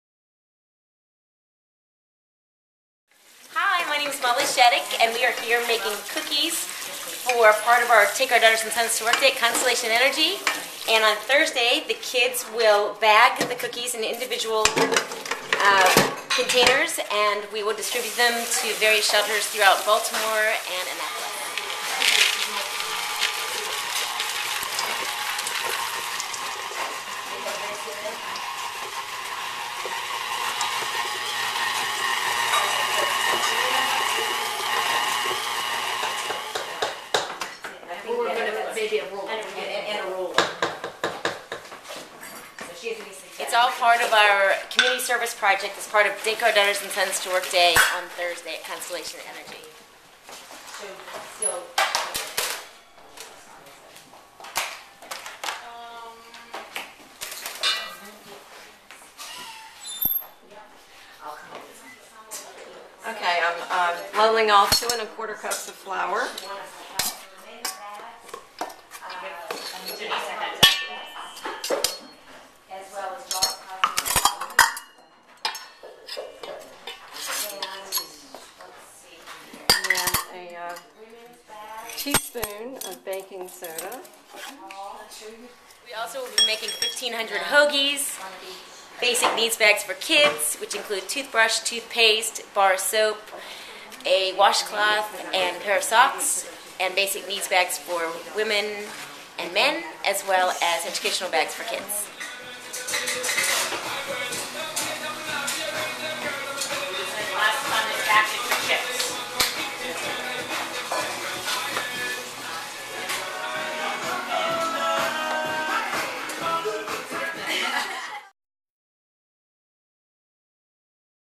Constellation Cooking With A Purpose
Molly Shattuck, wife of Constellation Energy CEO Mayo Shattuck, once again opened her home to a group of friends to cook up hundreds of cookies as a part of the Baltimore-based energy company's unique twist on "Take Our Daughters and Sons To Work Day". more than 150 children of Constellation Energy employees  Constellation Energy Kids  will gather in downtown Baltimore's Port Discovery to take part in community service activities organized by the company. provides children not only with the opportunity to shadow their parents during the workday, but also to learn more about the challenges facing society by giving back to their community through volunteer activities. Focused on the theme Warmth for the Homeless  Its Not Just About the Temperature. The cookies will be wrapped in individual bags and distributed to homeless shelters with hoagies made by the kids and other items.  WBAL video shot and produced by WBAL's Scott Wykoff (4/21/10).